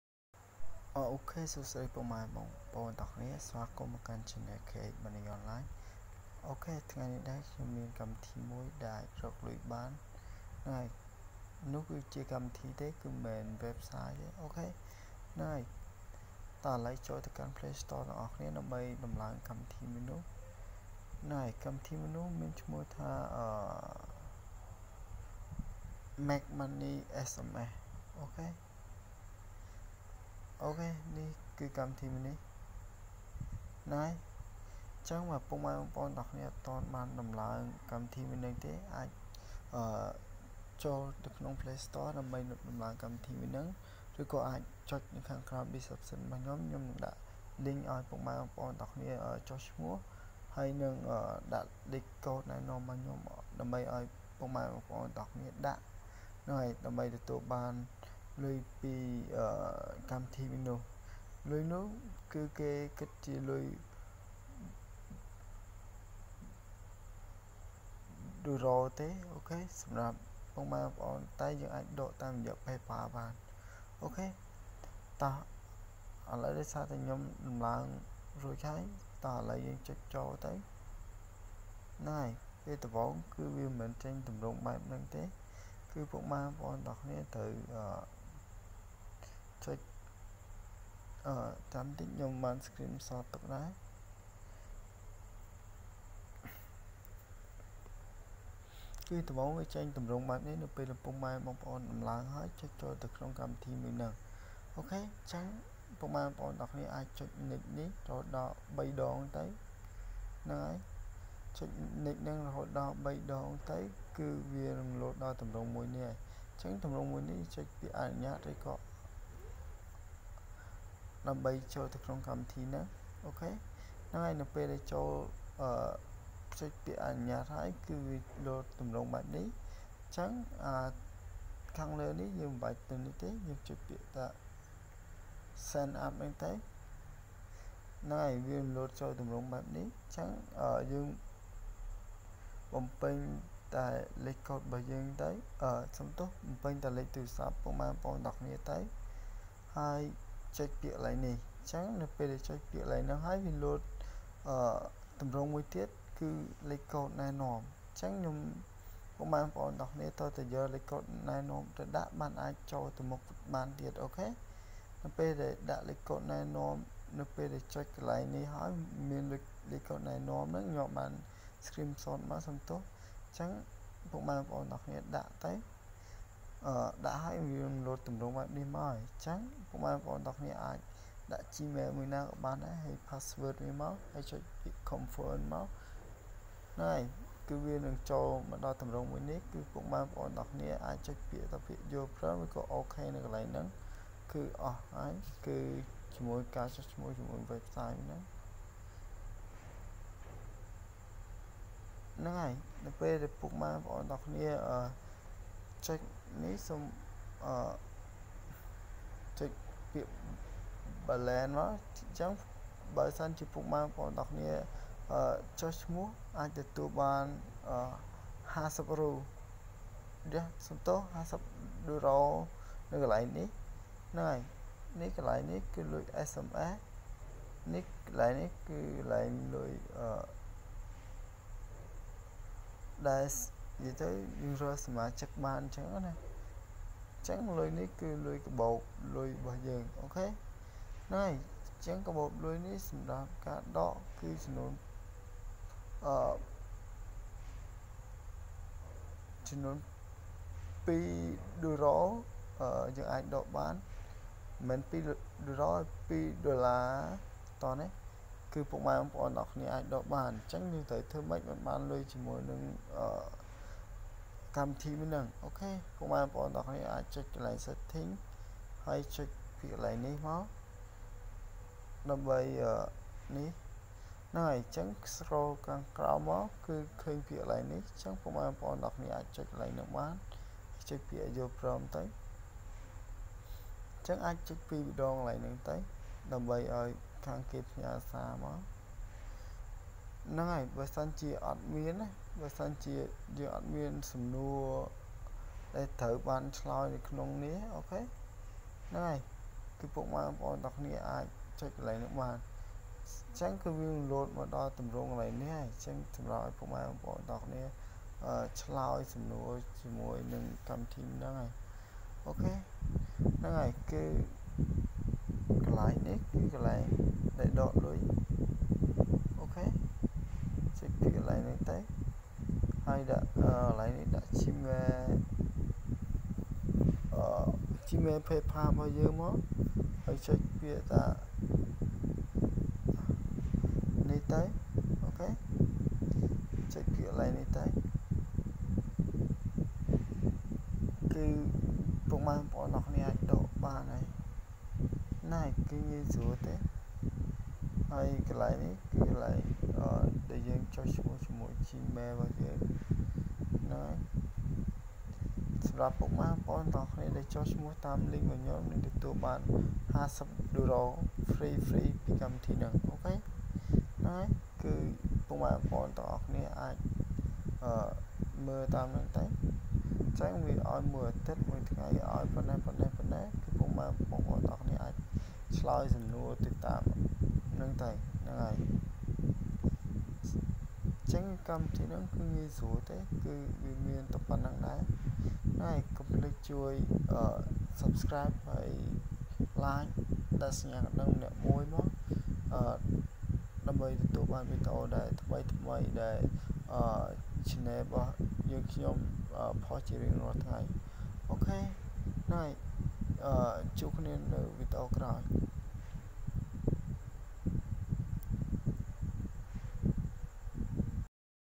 របៀបចុះឈ្មោះលេងកម្មវិធី Money SMS និងដាក់លេខកូដណែនាំនិងទទួលបានលុយ
👉នៅក្នុងវិដេអូនេះ ញុមចង់បង្ហាញរបៀបចុះឈ្មោះលេងកម្មវិធី Money SMS និងដាក់លេខកូដណែនាំនិងទទួលបានលុយ   ។ KH  Money Online

កូដណែនាំ 13E8D04B0D

      ពុកម៉ែបងប្អូនទាំងអស់គ្នាបាទ  សូមអរគុណ ។

# មើលហើយអនុវត្តតាម !